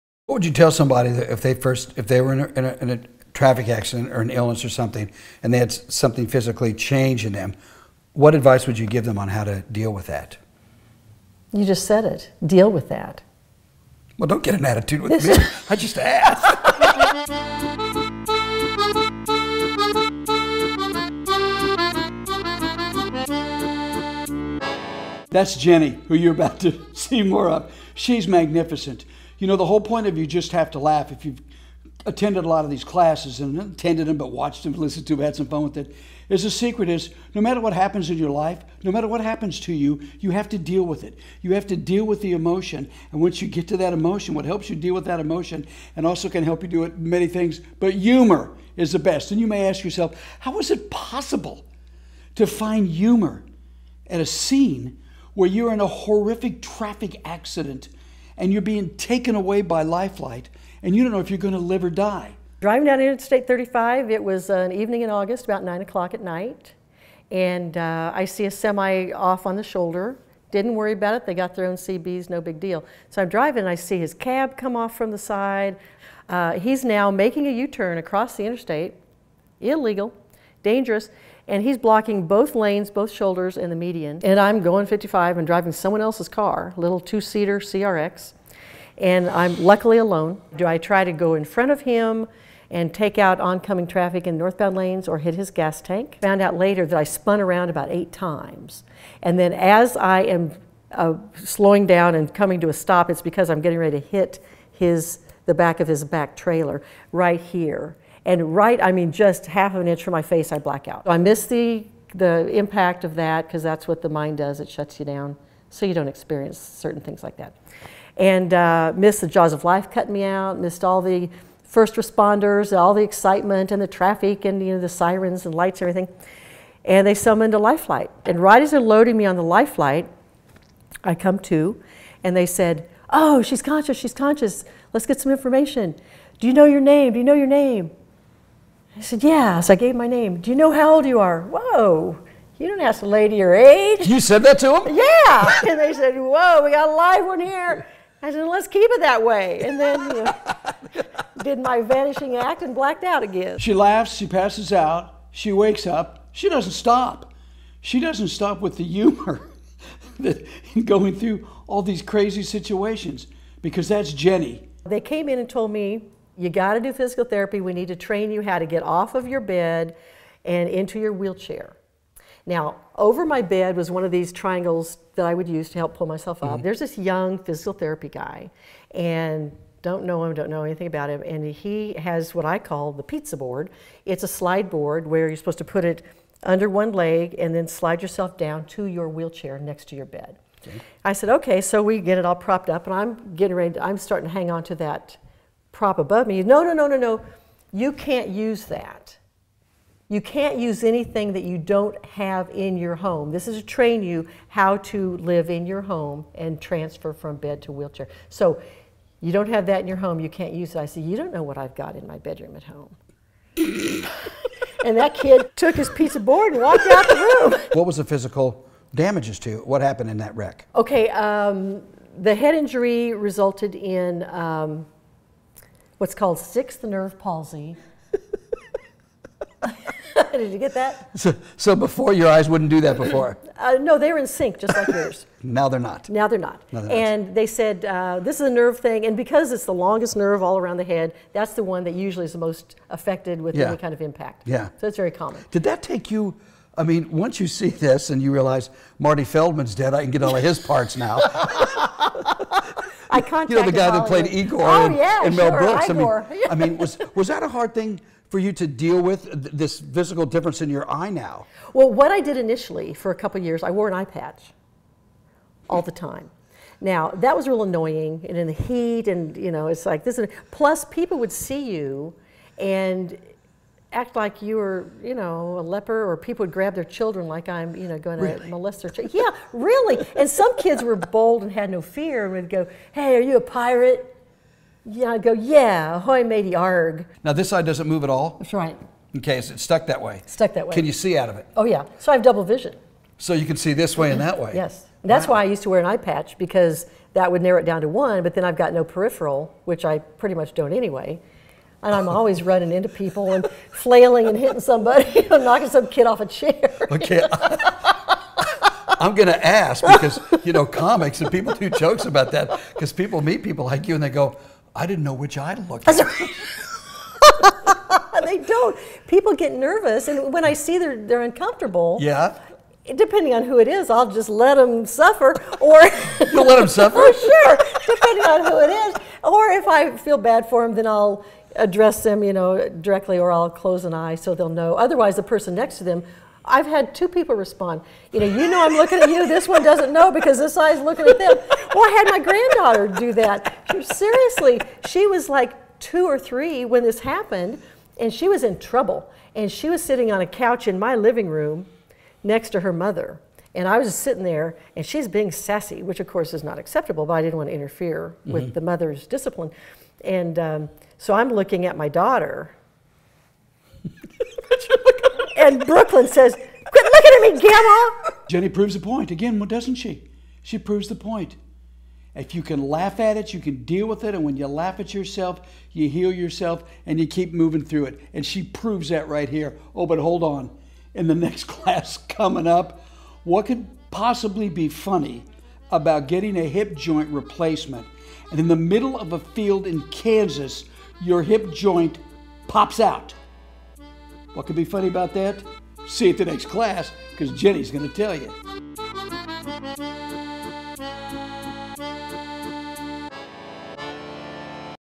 Nasterclass #50 Surviving a head on crash with Humor
Ginny is a one of a kind. She is funny, playful and insightful on how to get through serious physical injuries after a head-on collision. Her ability to laugh at herself are refreshing and brilliantly funny. You are welcome to share any of these classes with others so we can help heal the world - ONE LAUGH AT A TIME!

naster.com for more about David Naster and You Just Have To Laugh